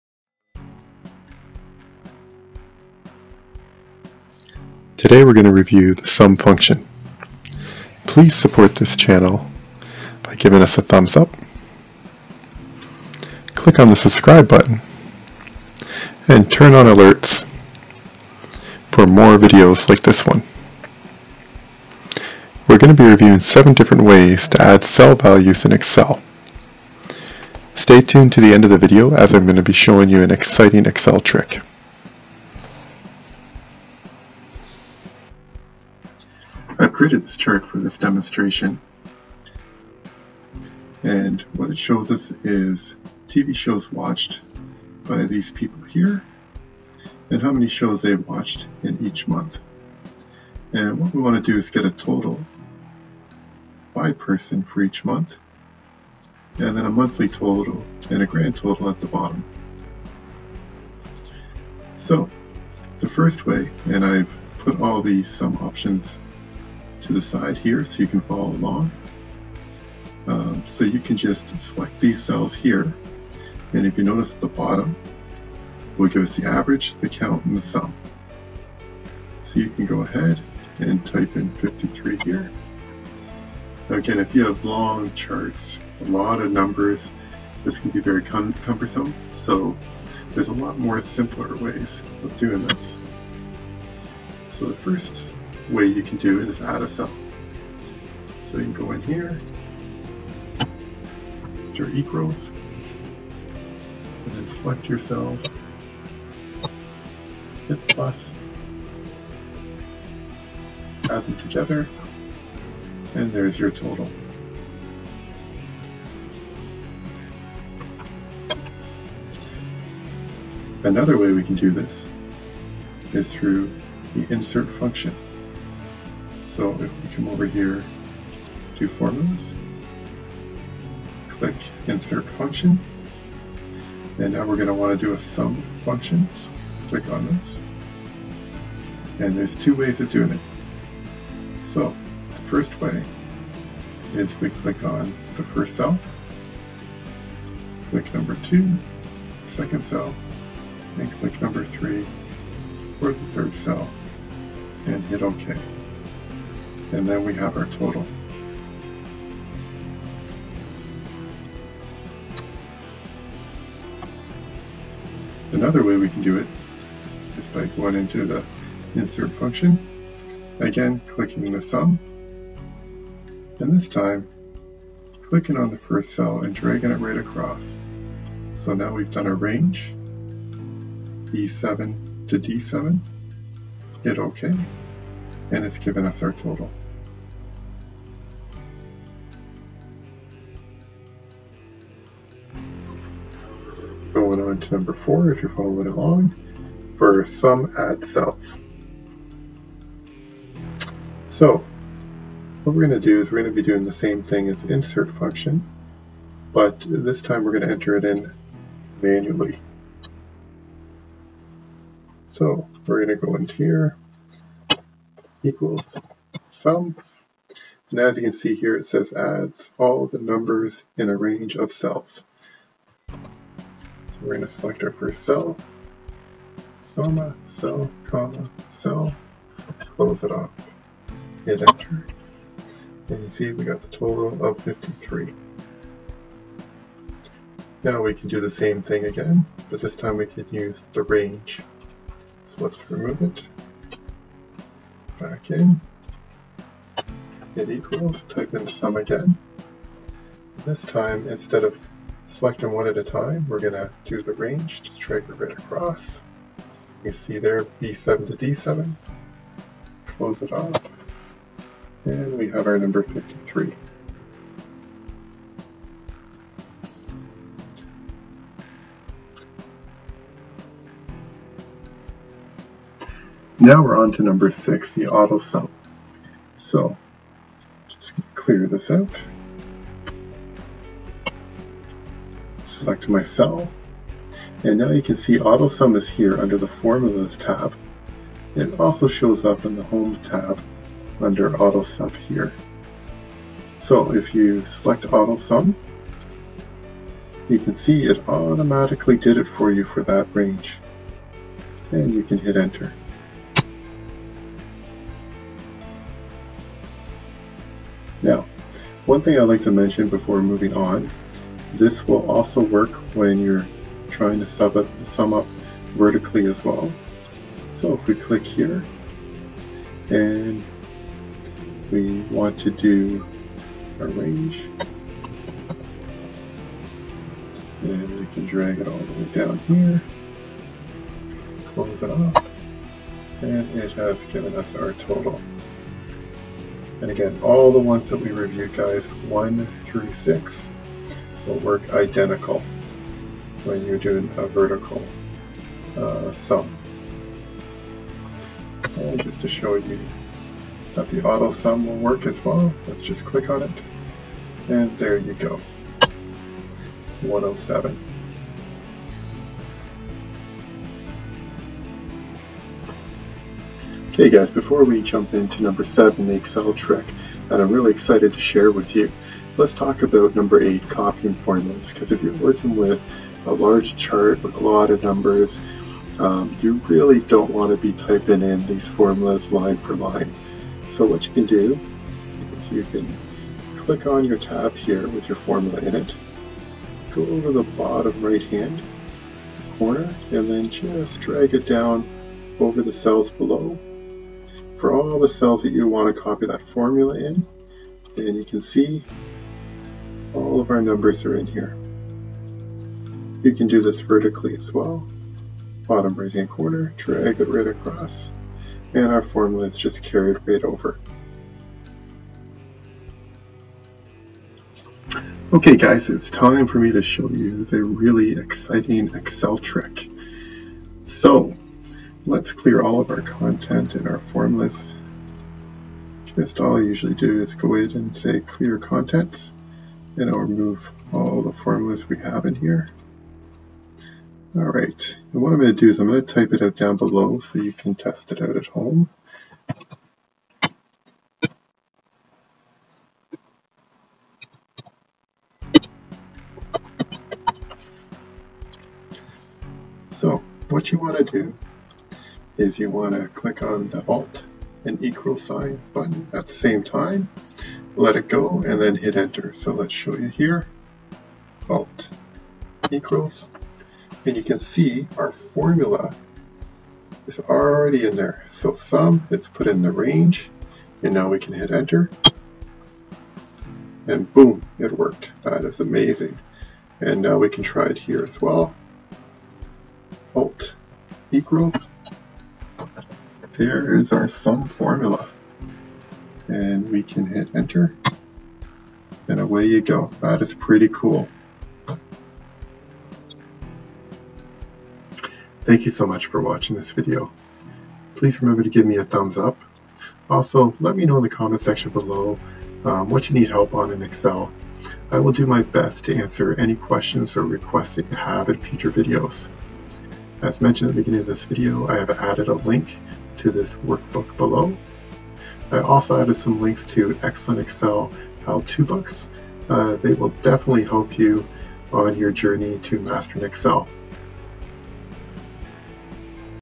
7 Ways to Add Cell Values in Microsoft Excel. Sum Function, Auto Sum Formula, Insert Function
Use the SUM function in Excel to sum a range of cells, an entire column or non-contiguous cells. To create awesome SUM formulas, combine the SUM function with other Excel functions.

Whenever you need to sum a single range of cells, whether a column, row or several adjacent columns or rows, you can have Excel AutoSum to automatically make an appropriate SUM formula for you.

GetExcel 2019 Bible